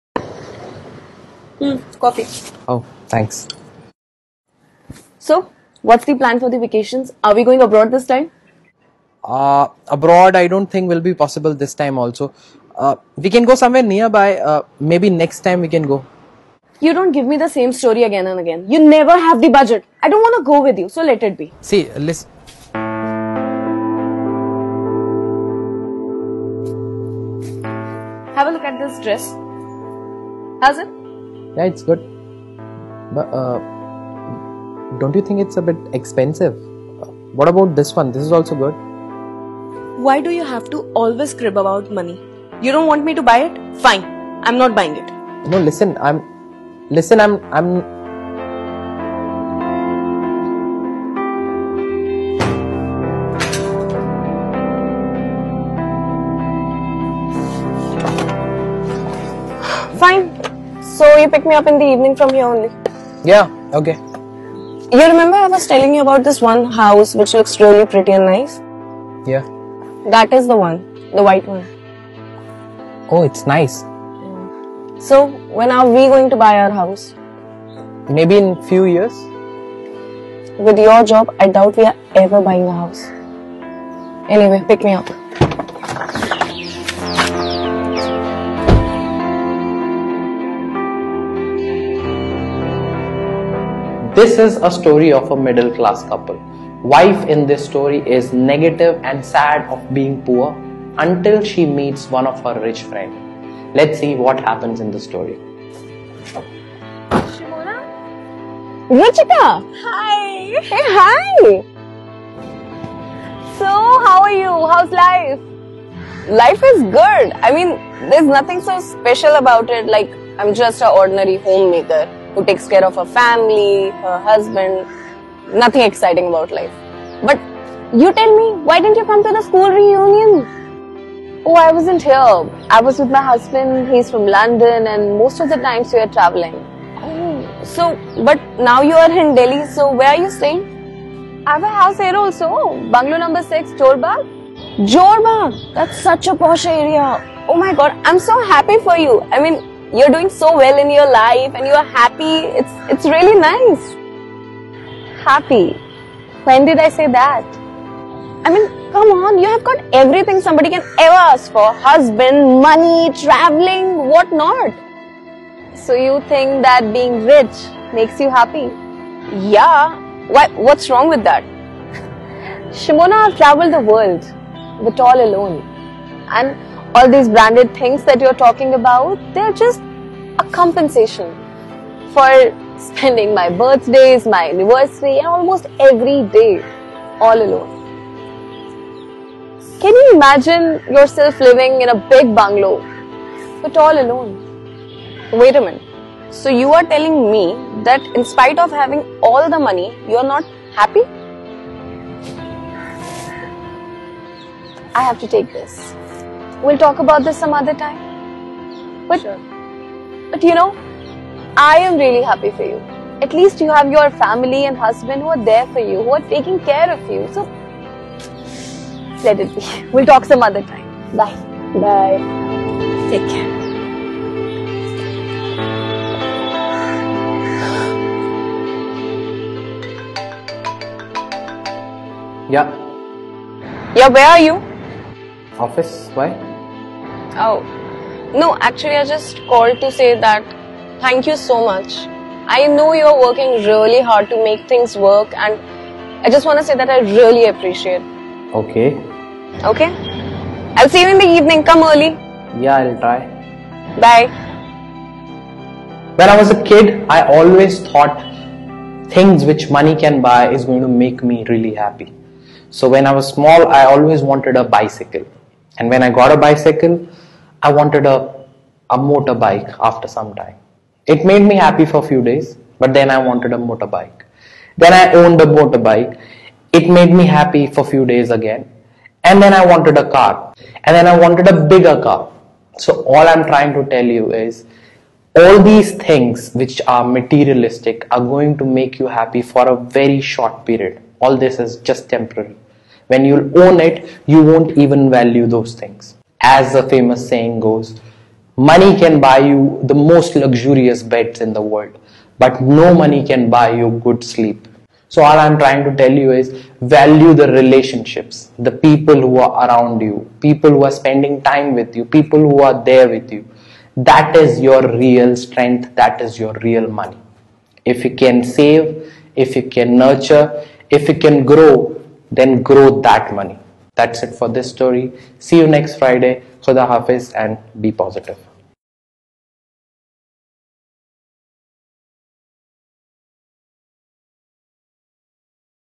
How to be happy ? What is happiness . She was sad for being poor until she Met a rich friend.
How to be happy ? What is happiness . She was sad for being poor until she Met a rich friend. Best motivational video